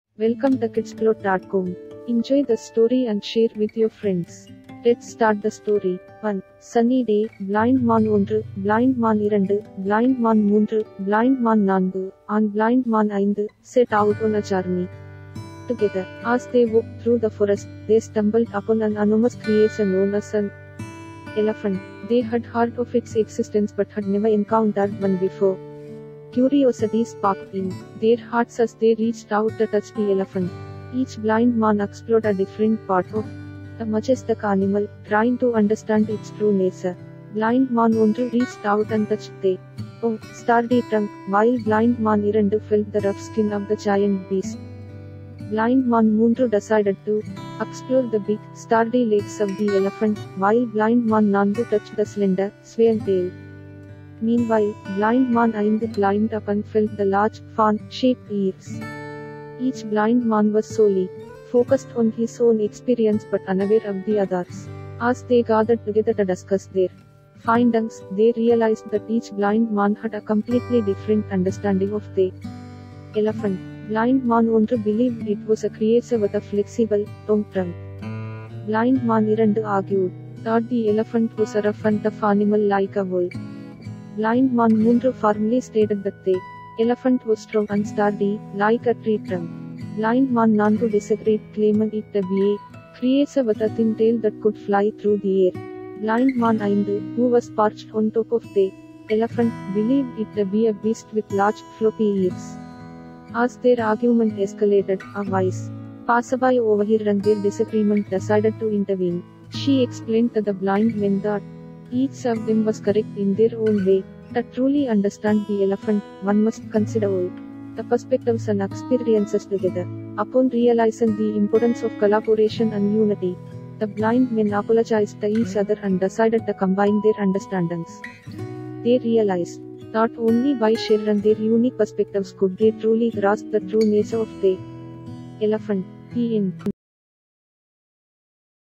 குருடர்கள் மற்றும் யானை | kidsplot.com
This video is generated by AI. to generate yours
 ஒரு வெயில் நாளில், ஐந்து பார்வையற்றவர்கள் ஒன்றாக பயணம் செய்தனர். காட்டில், அவர்கள் ஒரு யானை மீது தடுமாறினர். ஆர்வத்தைத் தூண்டியது, ஒவ்வொரு பார்வையற்ற மனிதனும் யானையின் வெவ்வேறு பகுதியைத் தொட முயன்றான். அவர்கள் தங்கள் கண்டுபிடிப்புகளைப் பற்றி விவாதிக்க ஒன்றாகக் கூடினர், மேலும் அவர்கள் ஒவ்வொருவருக்கும் யானையைப் பற்றி வெவ்வேறு புரிதல் இருப்பதை உணர்ந்தனர். ஒரு புத்திசாலியான வழிப்போக்கன் தலையிட்டு ஒவ்வொரு கண்ணோட்டமும் சரியானது என்று விளக்கினார். பார்வையற்றவர்கள் ஒருவருக்கொருவர் மன்னிப்பு கேட்டு தங்கள் புரிதலை இணைக்க முடிவு செய்தனர். தங்கள் பார்வையைப் பகிர்ந்து கொள்வதன் மூலம் மட்டுமே யானையை உண்மையாகப் புரிந்து கொள்ள முடியும் என்பதை அவர்கள் உணர்ந்தனர். முடிவு.